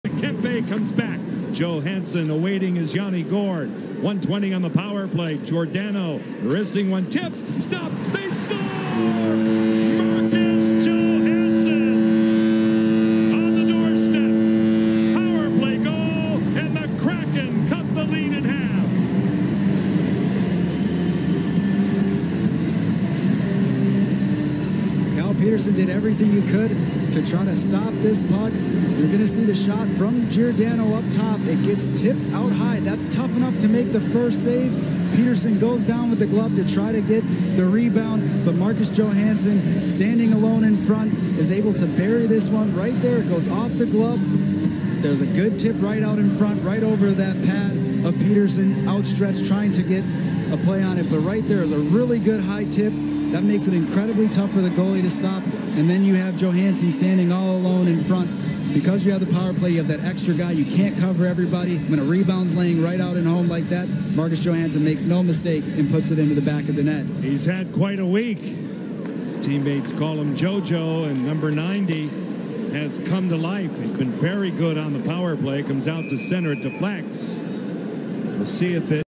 Marcus Johansson Gets The Kraken On The Board To Cut The Deficit In Half
Seattle get themselves on the board as Marcus Johansson in front gets this one to go on Cal Petersen to cut the deficit to one goal.